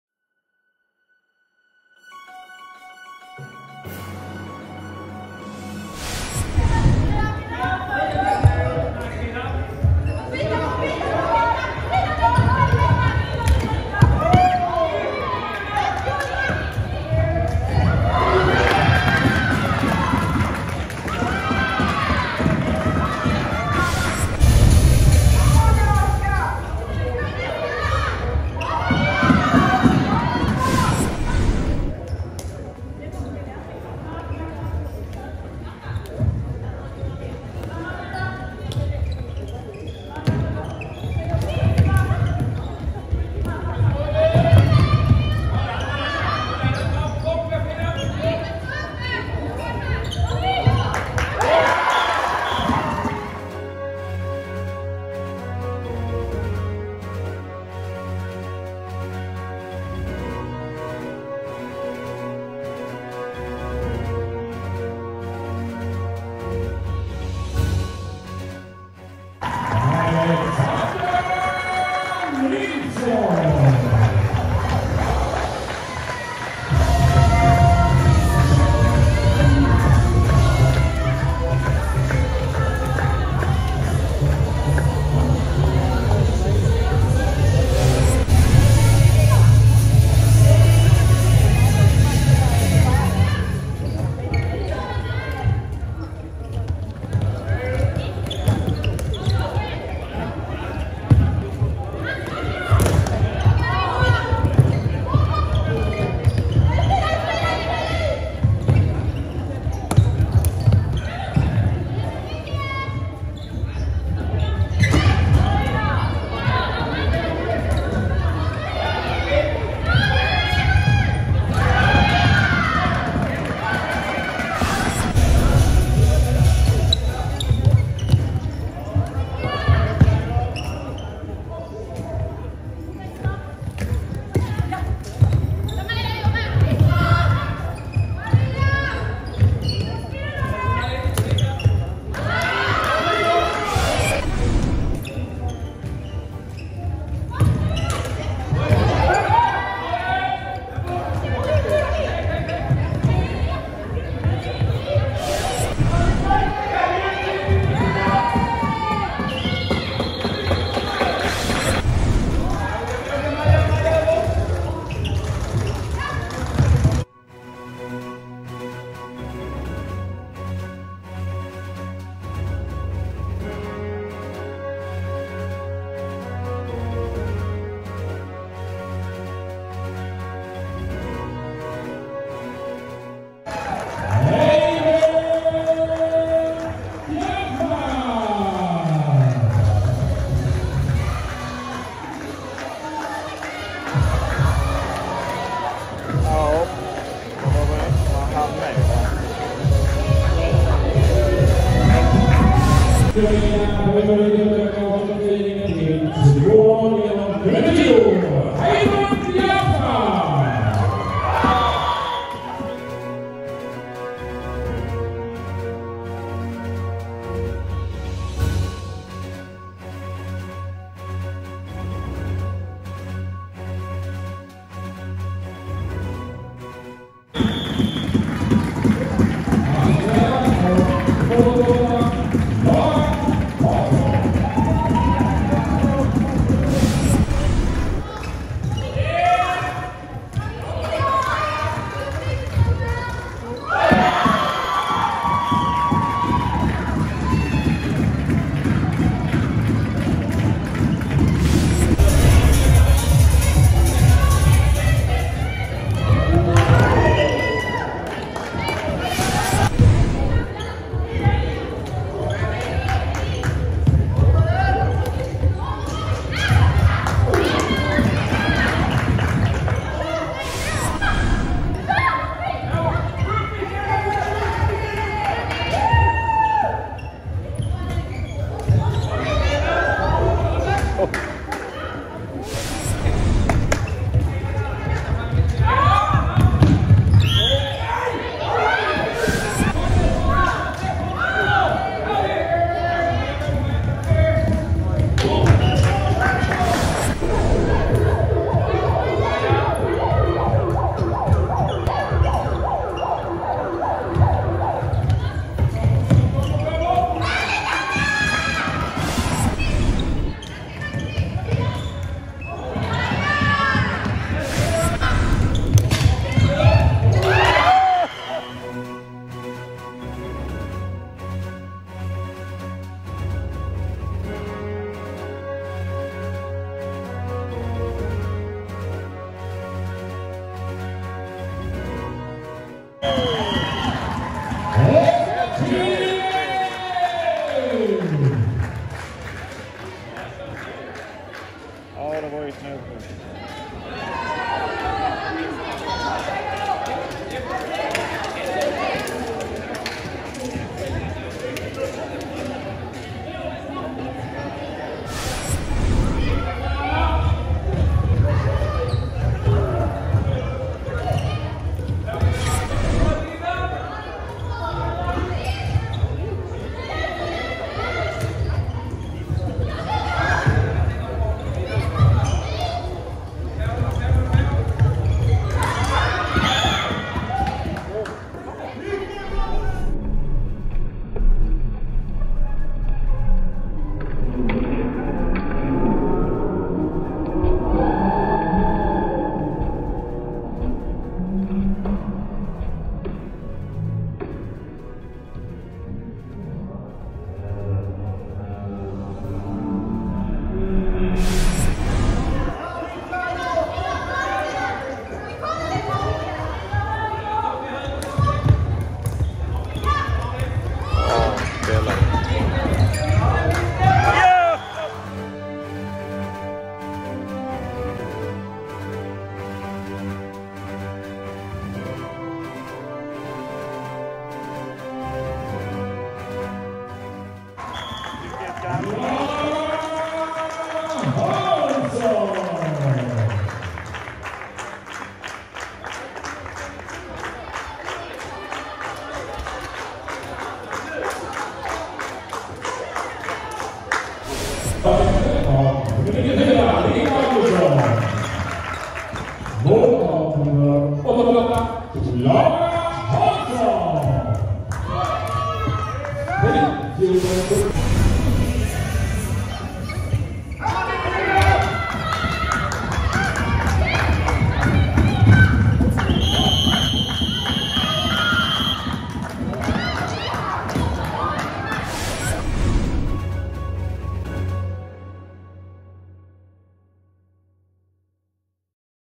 Highlights Dam.D1. Åstorp/Kvidinge IBS - Röke IBK 4-2
Det blev en spännande match där Åstorp/Kvidinge avgjorde sista perioden genom ett snyggt spelmål 4-2 av #88 Clara Hansson assist #94 Linda Augustsson.